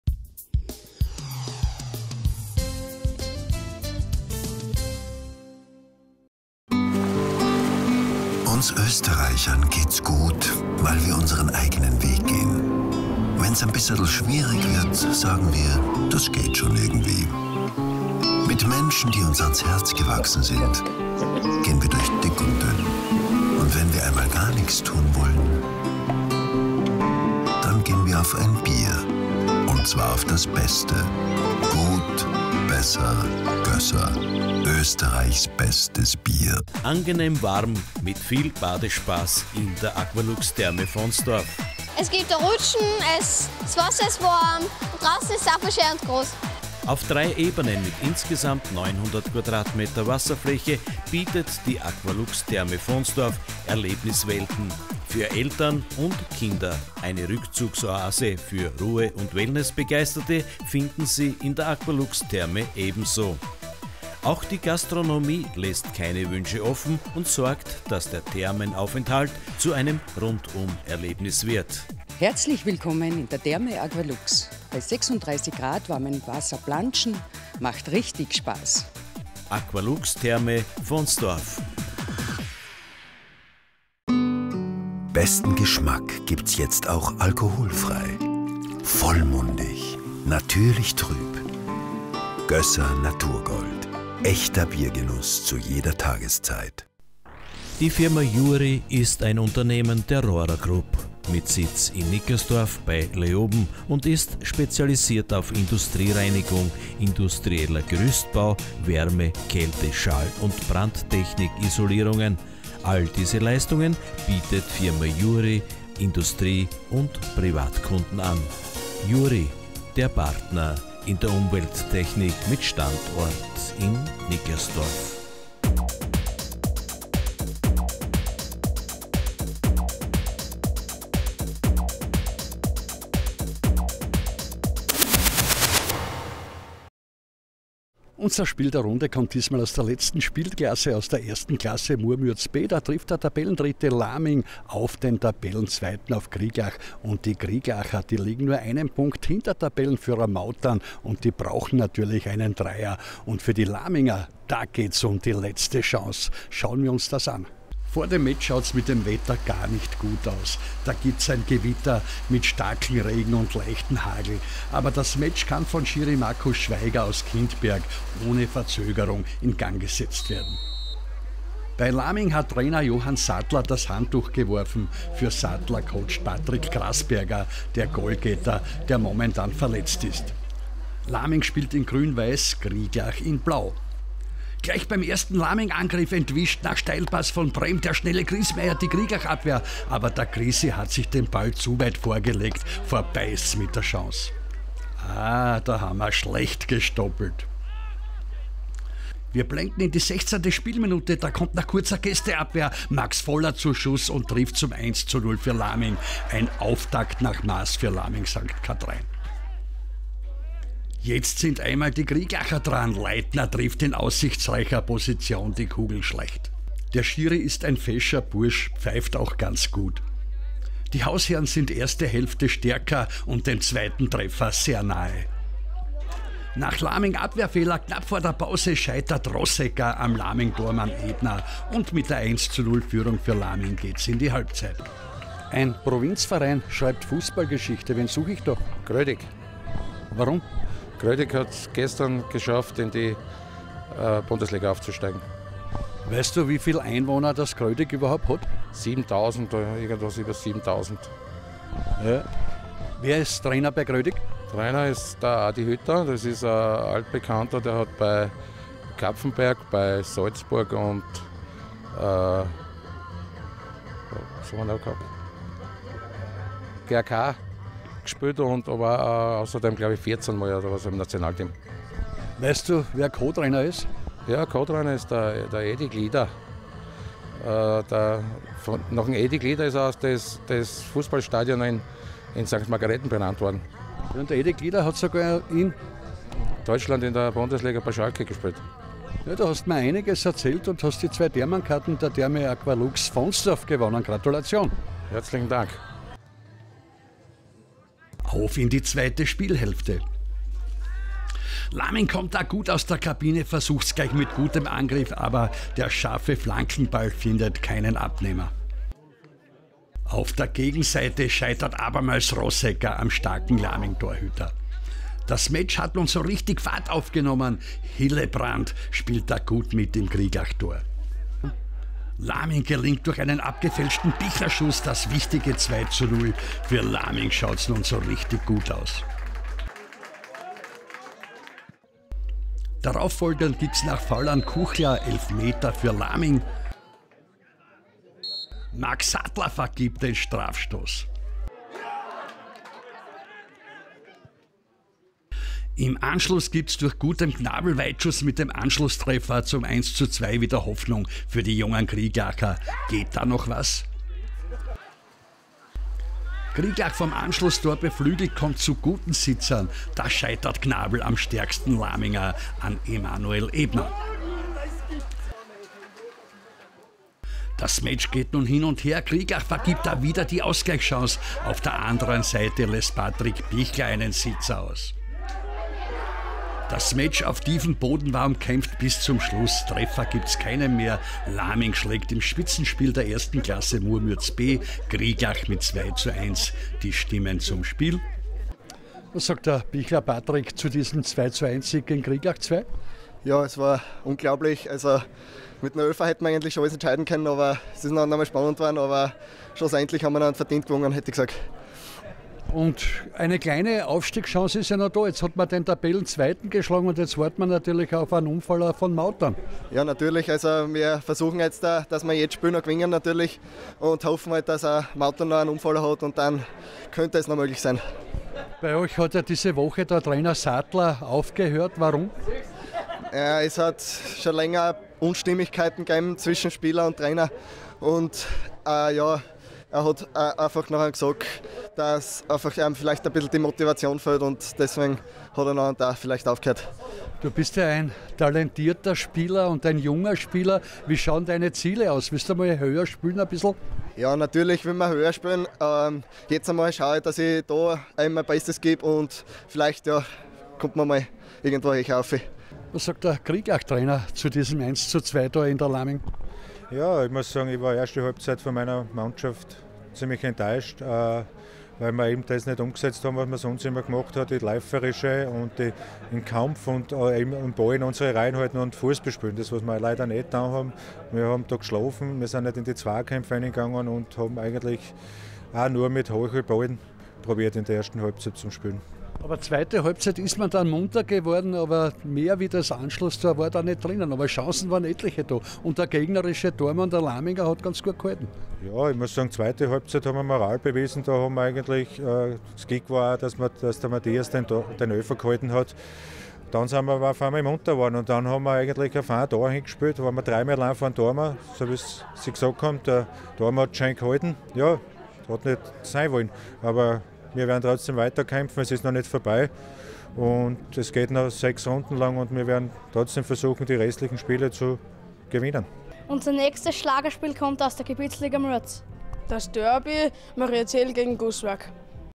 mematvo.at - KW 19 SPORTSTUDIO
Fußball: SV Laming gegen TUS Krieglach II